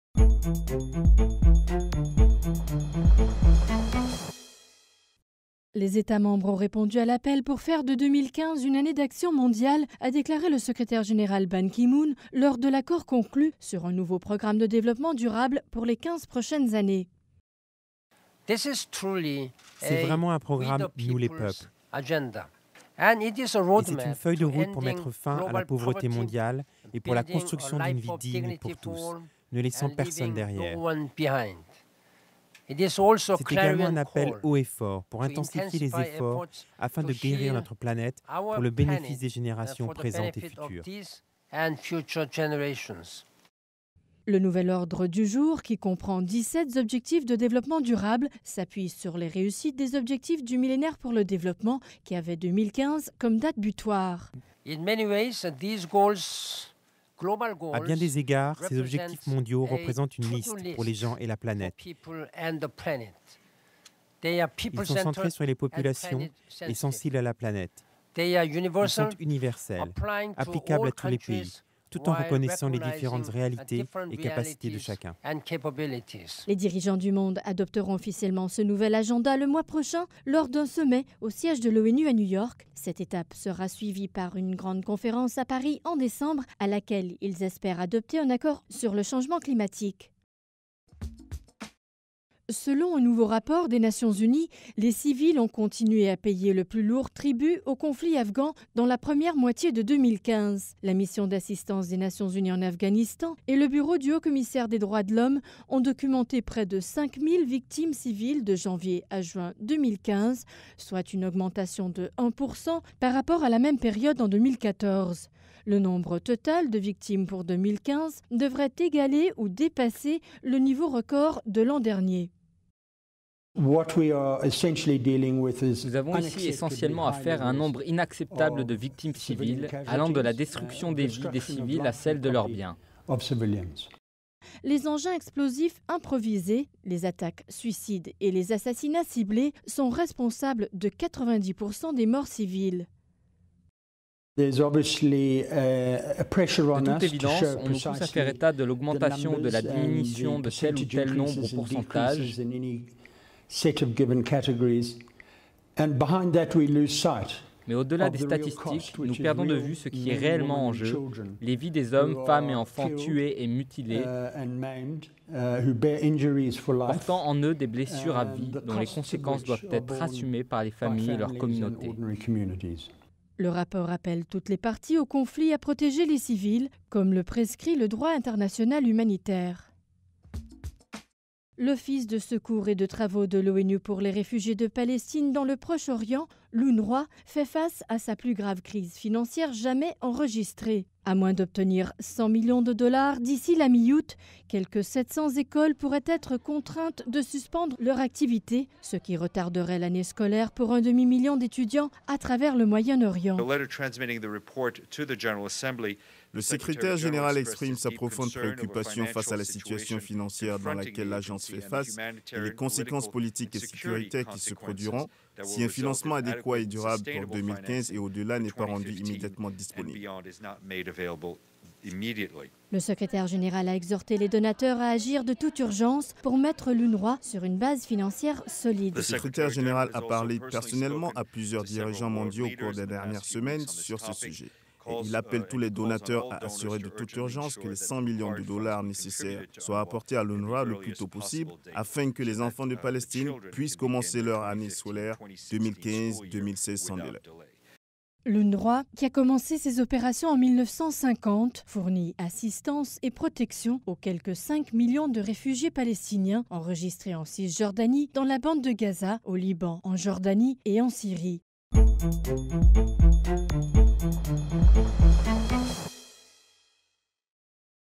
Revue de la semaine à l’ONU (3-7 août 2015)
Nations Unies - Revue de la semaine du 3 au 7 août  2015 – Faits saillants en images de l’actualité des Nations Unies avec Accord sur un nouveau programme de développement durable; Les pertes civiles afghanes atteignent un niveau record et l’UNRWA qui fait face à la pire crise financière.